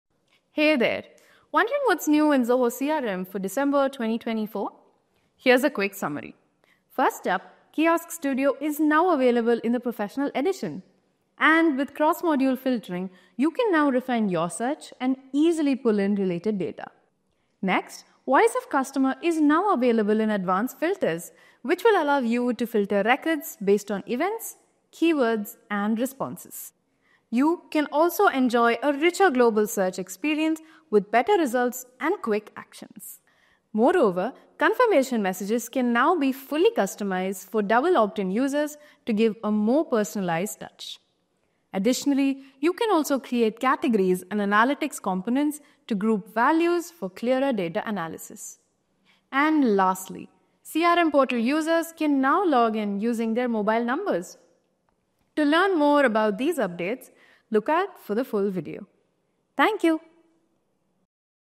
What's new in Zoho CRM? | December 2024
Zoho CRM now includes updated features.

From enhancements in Kiosk Studio's availability, Cross Module filtering, VoC, enhanced Global Search experience, enhancements for Double Opt-in users, Analytical Component, to logging in to CRM using mobile number.

Check out the full video to know more!